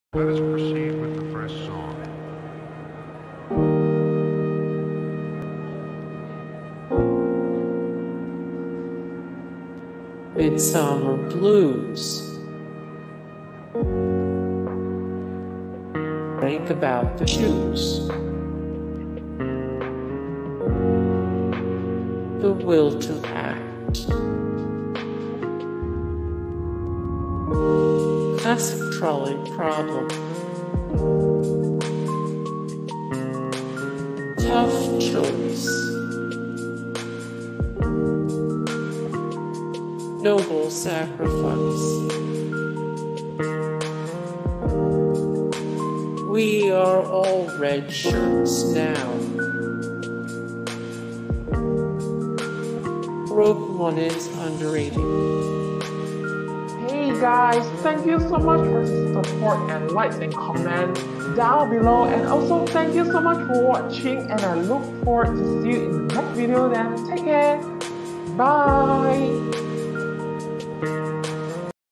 11 Actual Funny Posts From People Who Don'T Want Die From The Coronavirus To Help The Economy 🌠
Texas Lt. Gov Dan Patrick recently caused controversy by saying that older folks should be willing to essentially sacrifice their lives to Coronavirus in order to get the economy going again. Naturally the Internet didn't thnk this was a cool, or even rational, thing to say. Here are some of the best reactions.

0:00 - Intro
0:00:08 - Midsommar Blues
0:00:15 - Think About The Shoes
0:00:22 - The Will To Act
0:00:29 - Classic Trolley Problem
0:00:36 - Tough Choice
0:00:43 - Noble Sacrifice
0:00:50 - We Are All Red Shirts Now
0:00:58 - 'Rogue One' Is Underrated